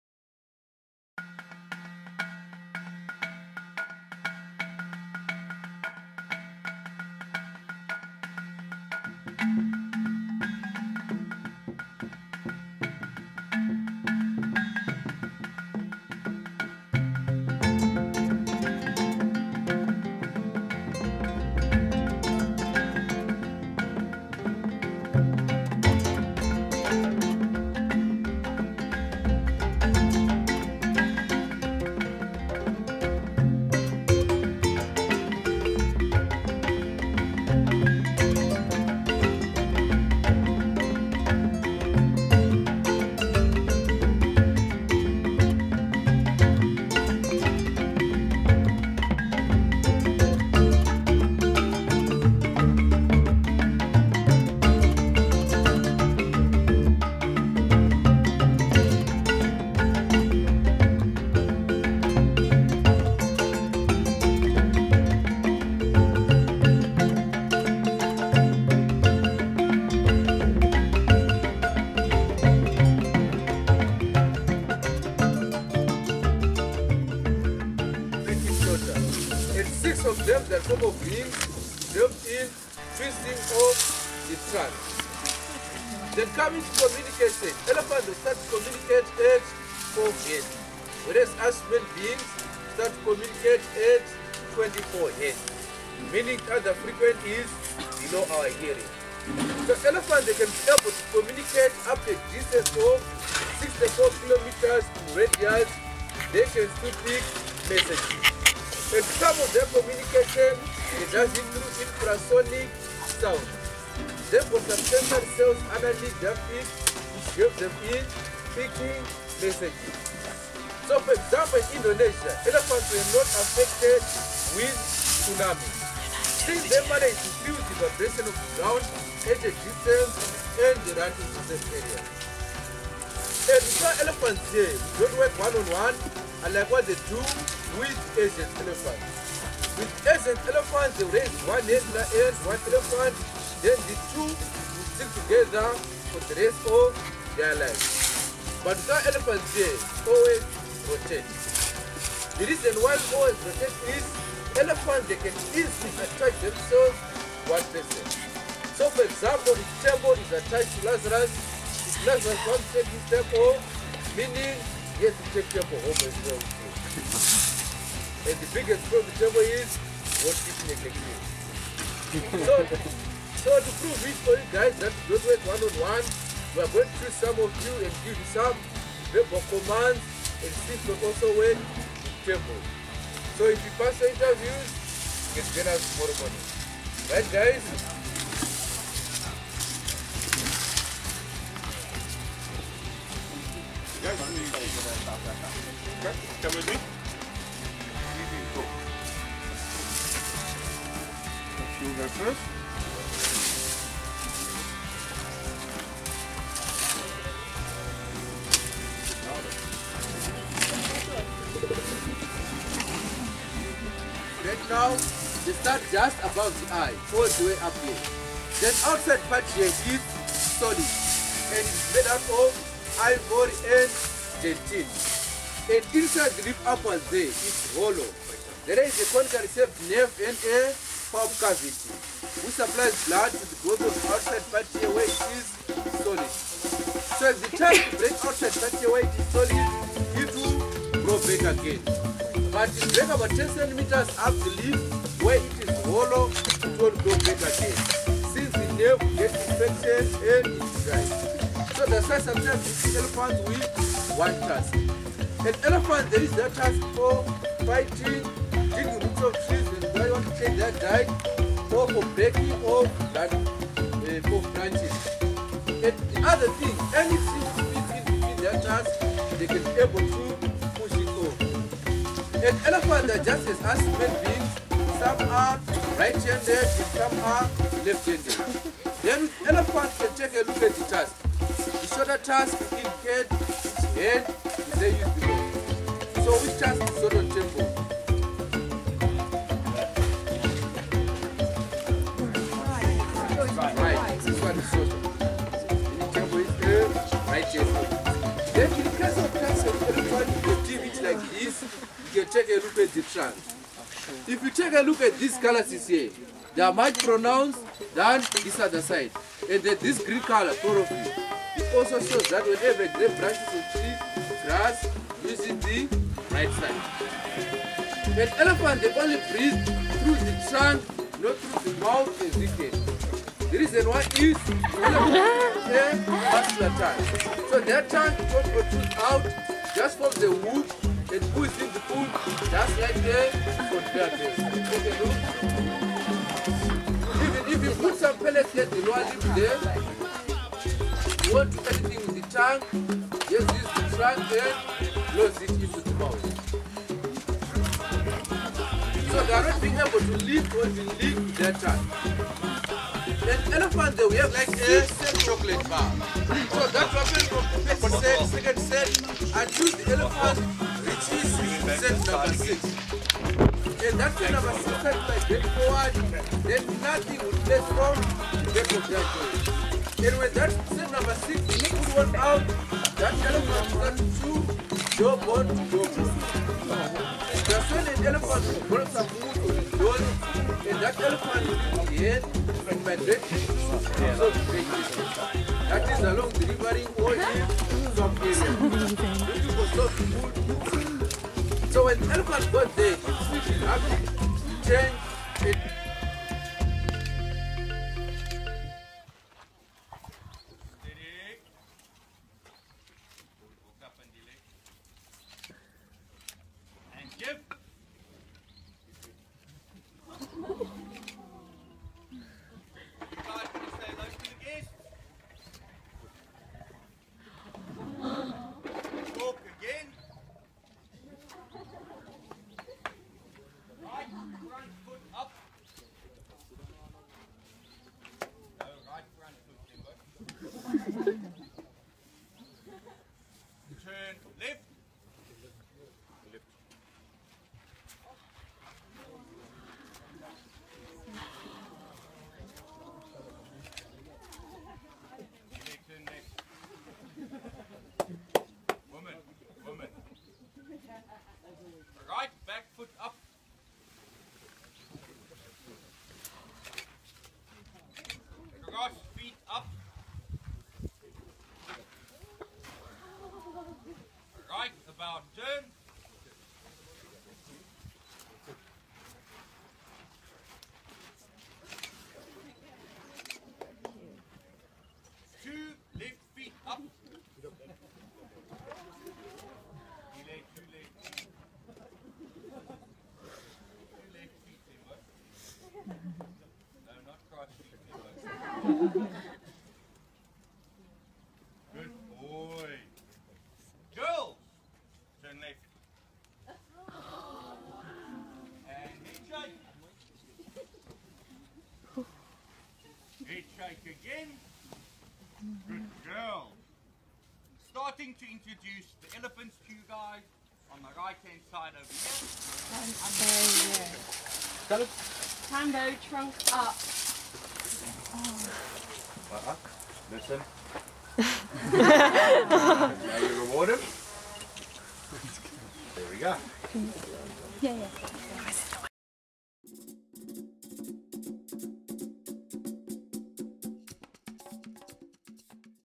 'Vlog' Elephant Whispers Center, Hazyview
A video of all of the footage I took at the Elephant Whispers Center in Hazyview, South Africa :)